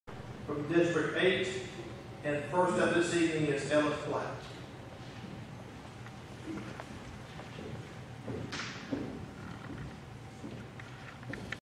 Franklin Bailey - Candidate Introduction @ Candidate Forum - 2014
Valdosta Lowndes County Chamber of Commerce Candidate Forum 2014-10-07